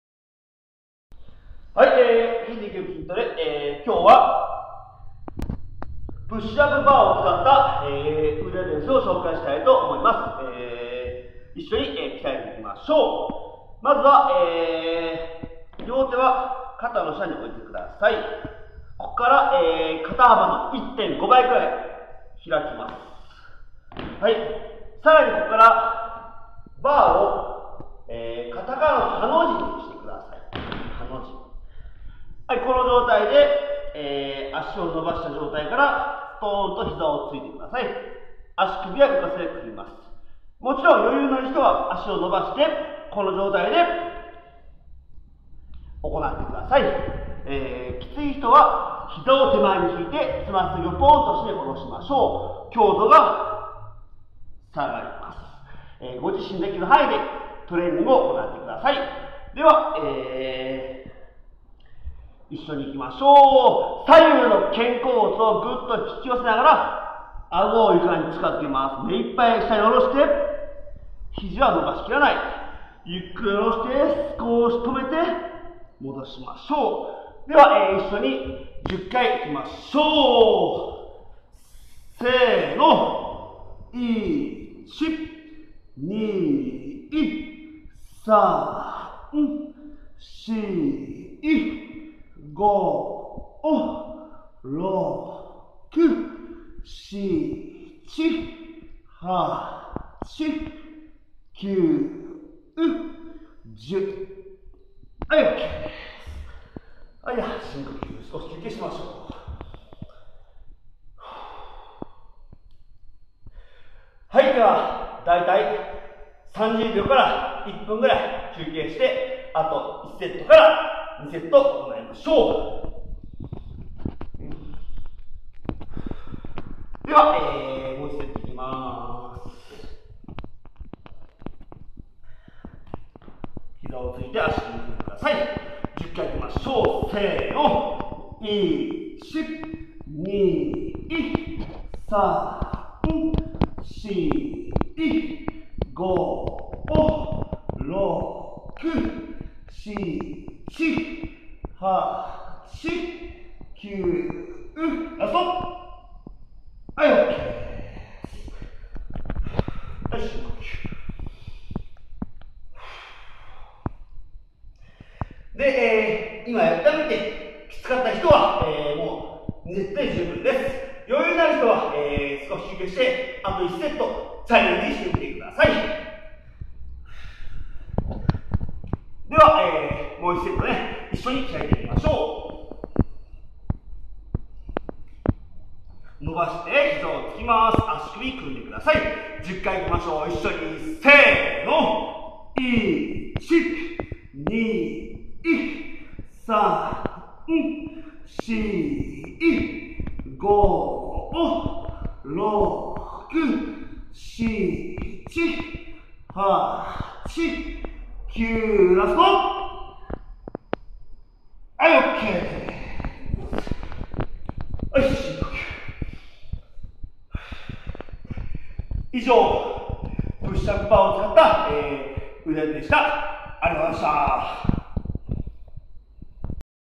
強度★★★【プッシュアップバーを使った腕立て伏せ】（胸のトレーニング）
健康運動指導士・中井良二が自宅で出来る簡単なトレーニングを紹介していくシリーズです。滑舌悪いです(笑)
今回は市販のプッシュアップバーを使った腕立て伏せです。フォームは基本通常の腕立て伏せと同じですが、バーはカタカナのハの字に向けて行います。肩への負担が軽減され、より胸への刺激が高まります。
10回を2～3セット。キツい方は膝を手前に寄せてつま先を下ろして行いましょう。余裕のある方は足を伸ばしてチャレンジしてみて下さい。